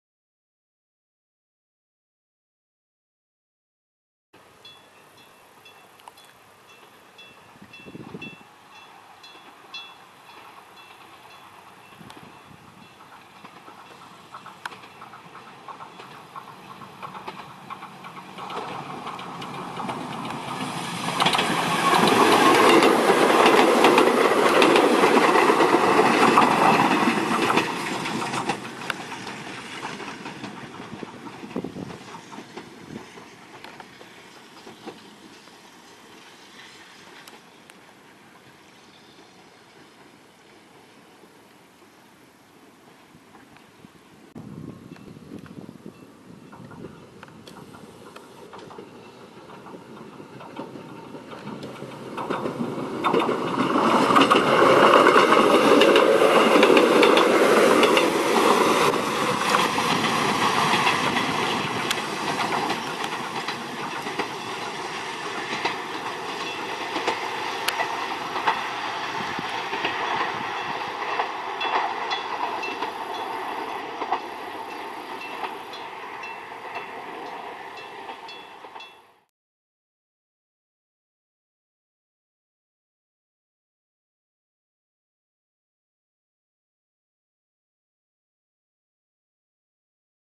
初秋の津軽路を走る,弘南鉄道・弘南線②.
初秋の津軽路を走る、弘南鉄道・弘南線。
平賀駅と館田駅の間にて撮影。